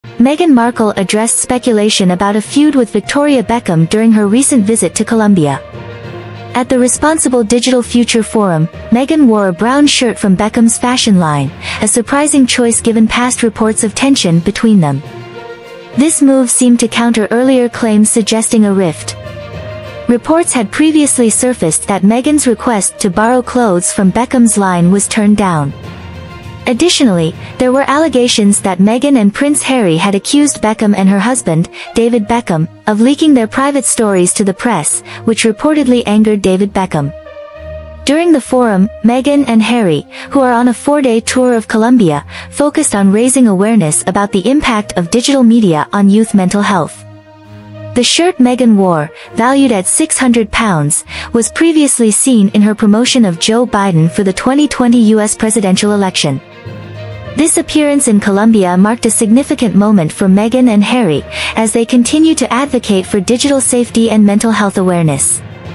Meghan Markle Clears Up Victoria Beckham Feud Rumors During Colombia Visit!
In this video, we dive into Meghan Markle's recent appearance at the Responsible Digital Future Forum in Colombia, where she took a surprising step to address ongoing feud rumors with Victoria Beckham. Wearing a brown shirt from Beckham’s fashion line, Meghan seemingly put speculation to rest about any discord between them.

We’ll explore:

The significance of Meghan’s choice to wear Beckham’s shirt and its potential implications for their relationship.
The backstory behind the rumored feud, including claims that Meghan’s request to borrow Beckham’s clothes was denied and accusations of media leaks involving David Beckham.
Highlights from Meghan and Prince Harry’s four-day tour of Colombia, where they focused on the impact of digital media on young people's mental health.
A look back at Meghan’s previous use of the £600 shirt during her support for Joe Biden in the 2020 U.S. presidential elections.
Join us as we unpack the latest developments and what this might mean for Meghan, Victoria Beckham, and the broader context of their public lives.